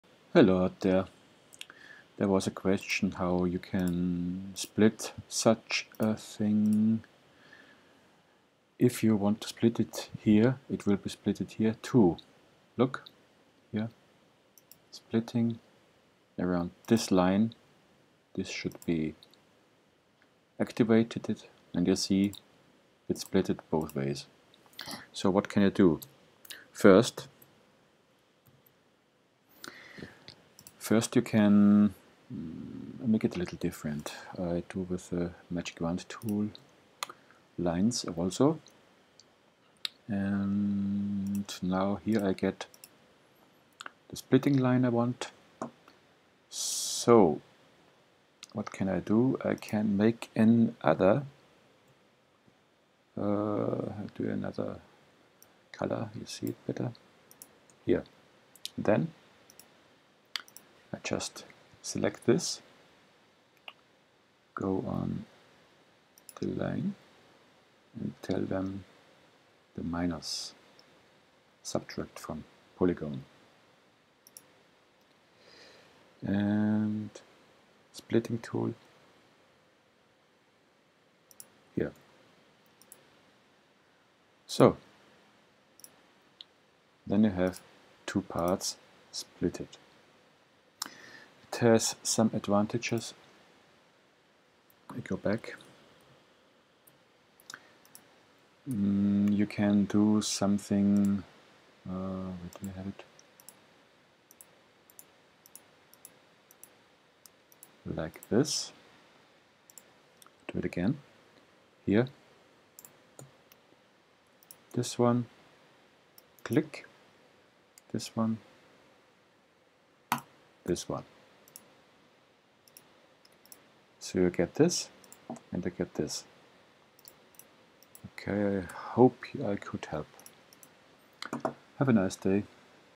Archicad 24 complexer splittings
How to split polygonal forms in a complexes Way than with the normal splitting-tool